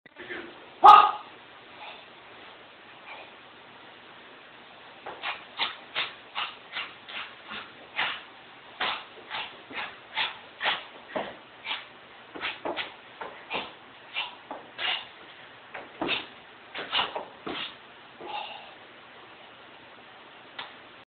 Ching Nin Kuen Doy Dar
Two person routine with Elizabeth Tran & Kelan Hightower